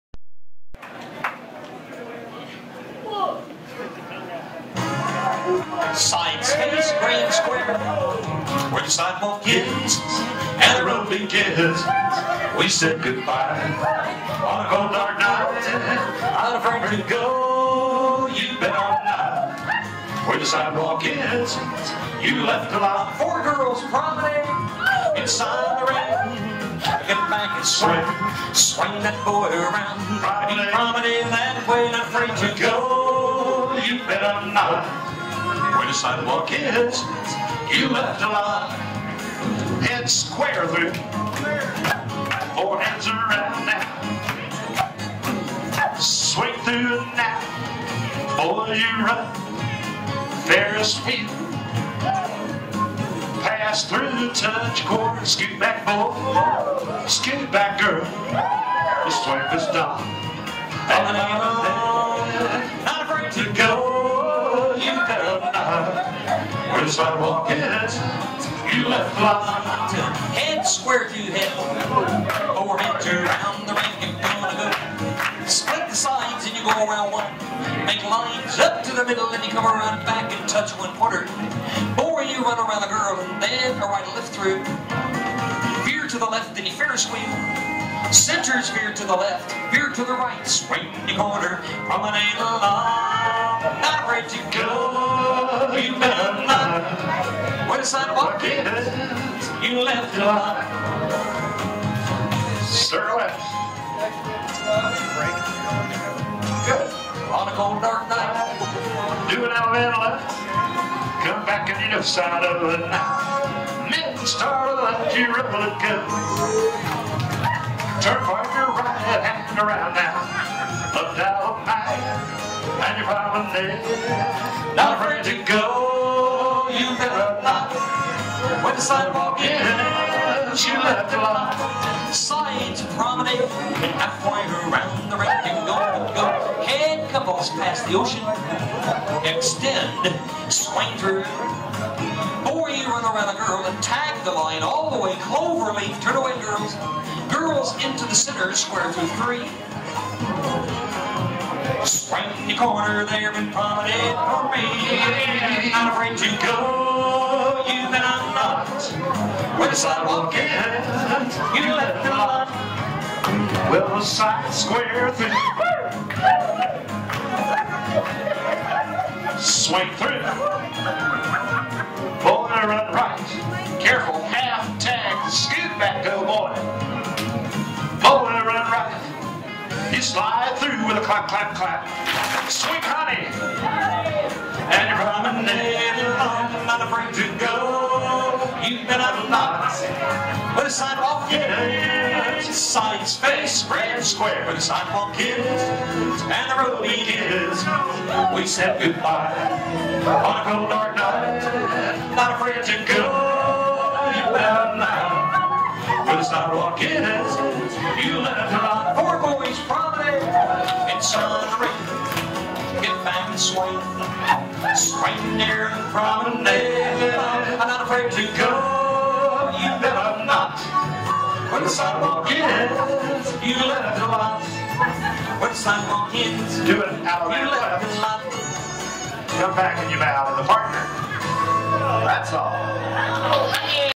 Square Dancing in Kansas City, Missouri Modern Western Style with Tom Roper VIDEO0094.3gp
Square Dancing in Kansas City, Missouri Modern Western Style with Tom Roper & Jay Krebs square dance callers